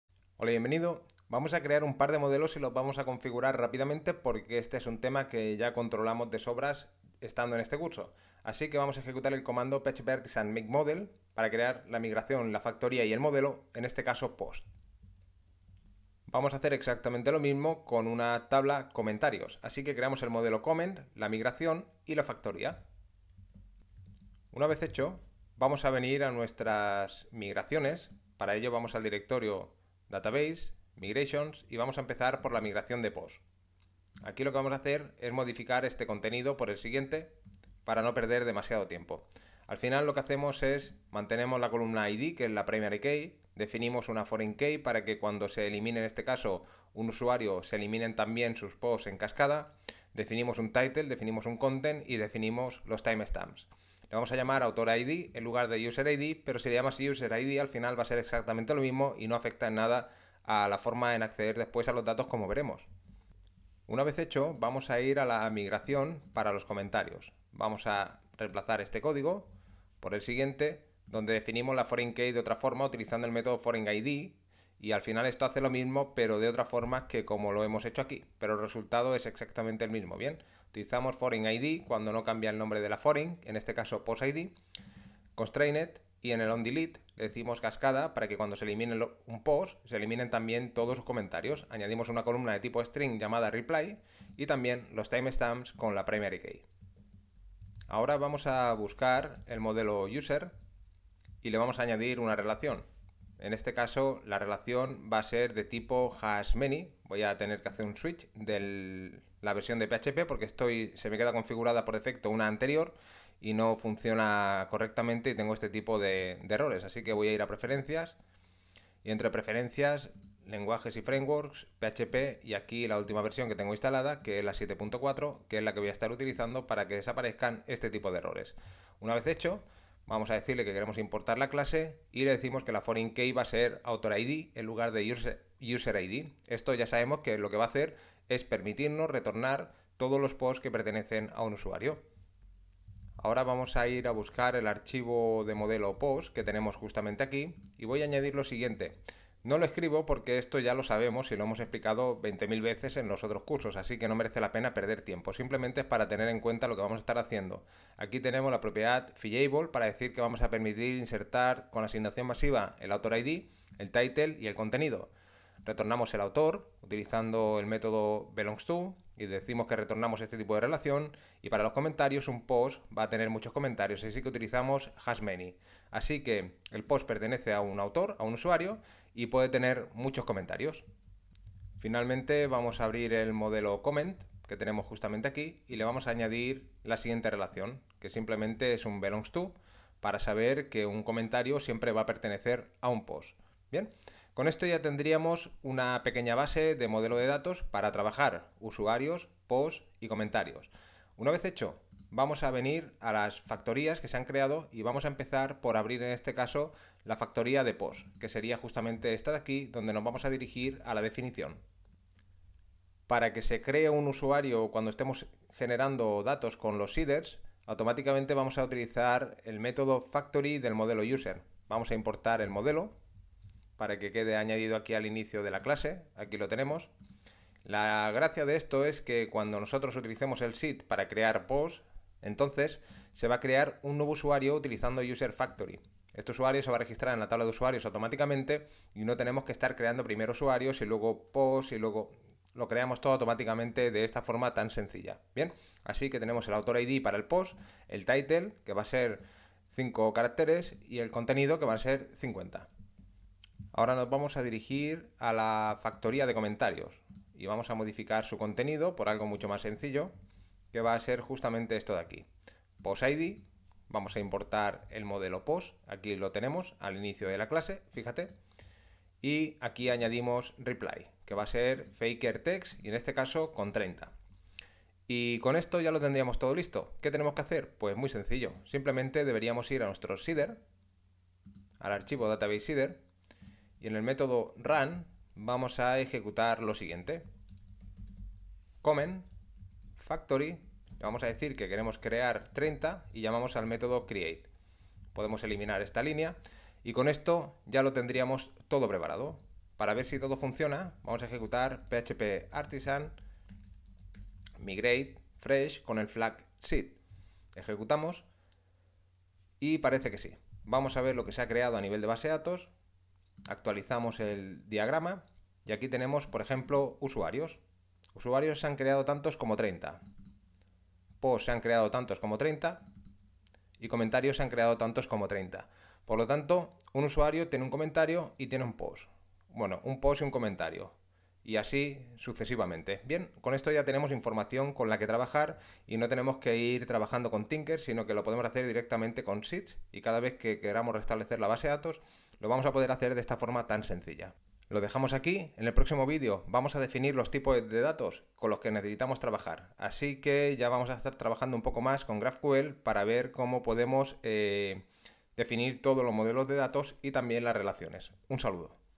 #3 Modelos, Migraciones, Factorías y Seed para probar servidor GraphQL
🔥 ¿Quieres seguir aprendiendo GraphQL en Laravel? 🔥